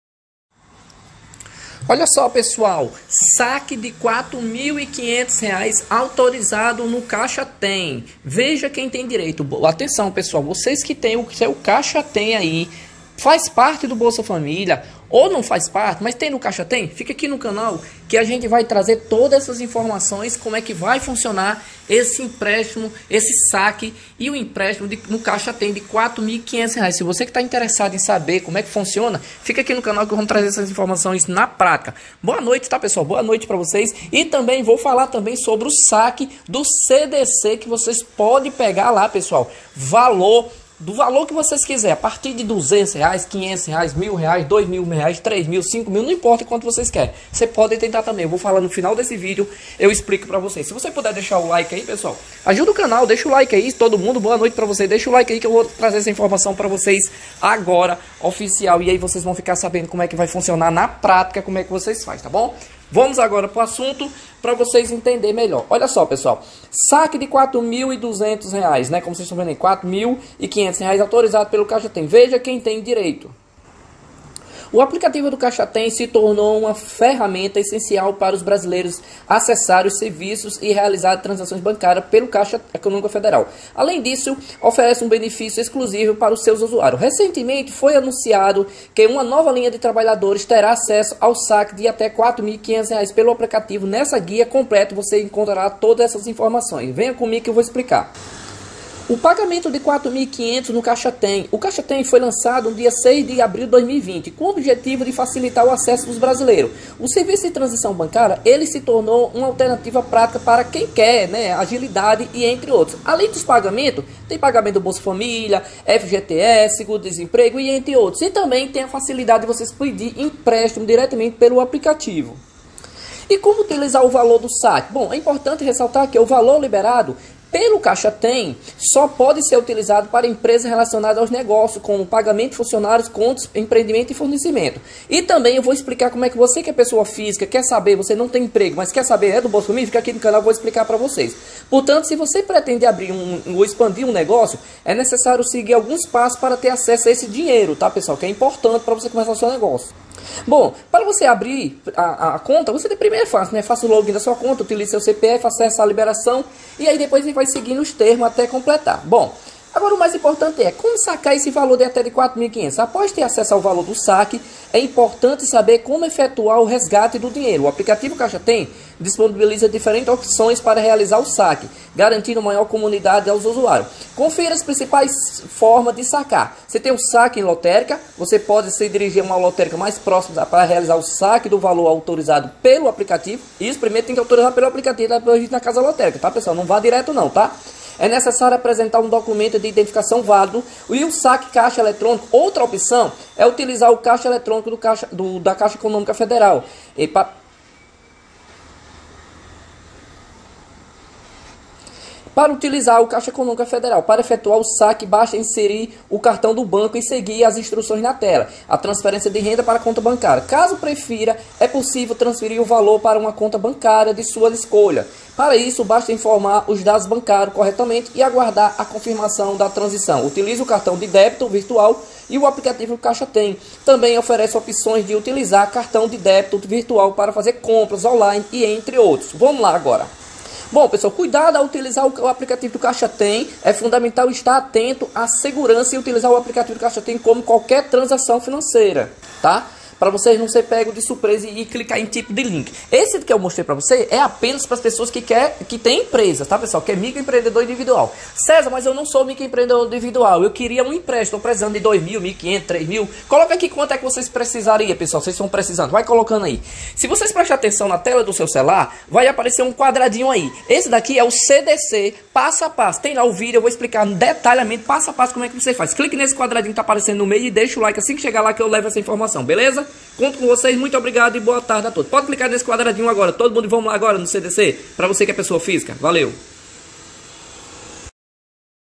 R$ 4.500 SERÁ LIBERADO PELO CAIXA TEM; CONFIRA AQUI
CAIXA LIBERA EMPRÉSTIMO CDC PARA TODOS: veja o passo a passo

Notícias, informações e opinião sobre os assuntos mais relevantes

ABRAÇOS E FIQUEM COM DEUS